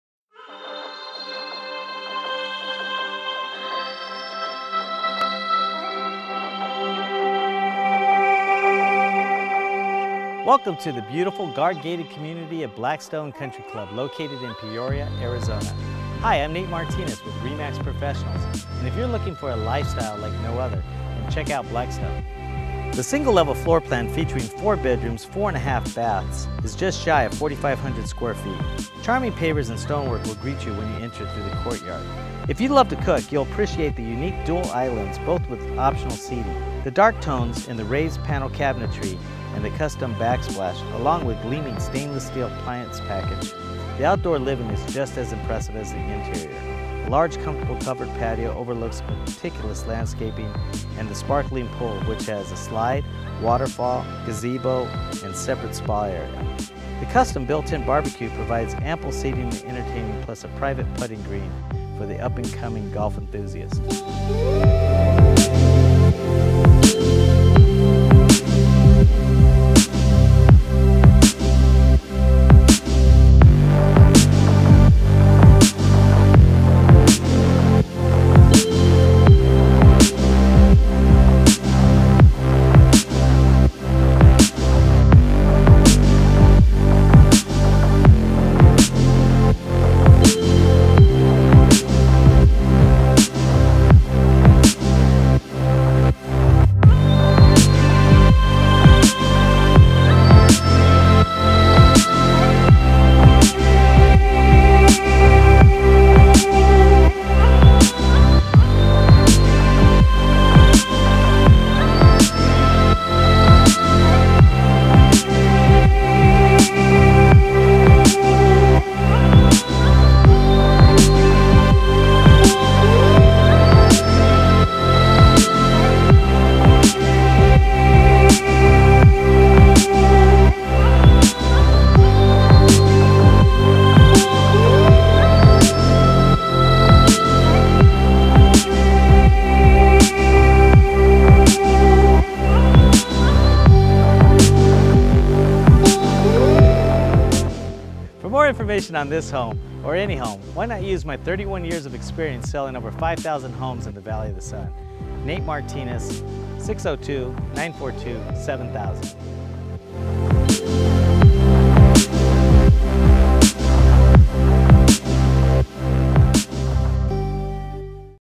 SOLD!! 30907 N 120th Avenue Blackstone Country Club
Upscale amenities abound in this Cachet Home, located in the desirable guard gated community of Blackstone Country Club. The single level floor plan features 4 bedrooms, 4.5 baths and is just shy of 4,500 square feet.

Nate Martinez
RE/MAX Professionals
NatesHomes.com